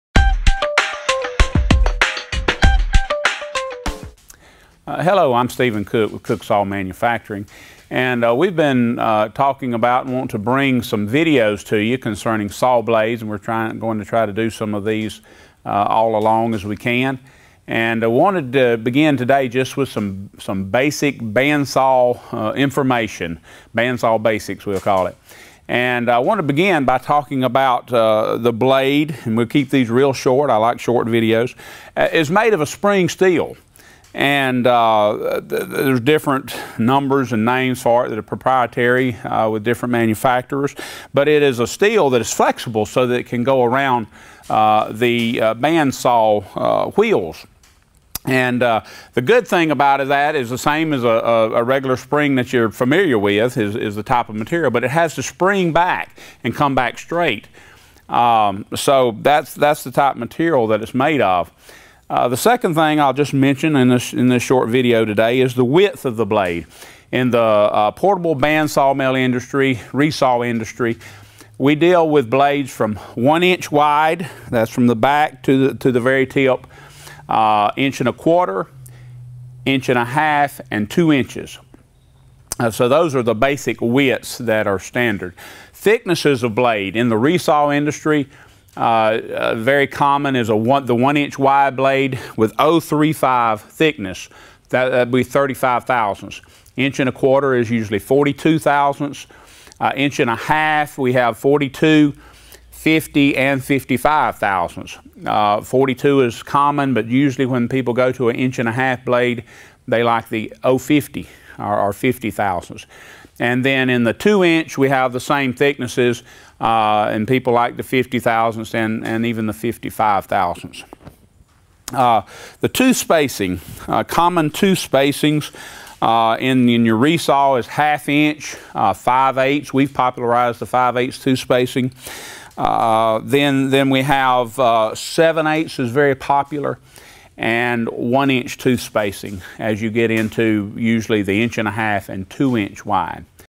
Sawmill Bandsaw Blade Basics Part 1 - Width, Thickness, and Tooth Spacing
Part one of a multi-series of dealing thin kerf bandsaw blades used on portable sawmills and resaws.
This video solely deals with the very basics of a bandsaw blade - width, thickness, and tooth spacing.
Additional videos will follow that go into greater depth dealing with each of these characteristics as well and many other aspects of bandsaw blades.
or call us toll free at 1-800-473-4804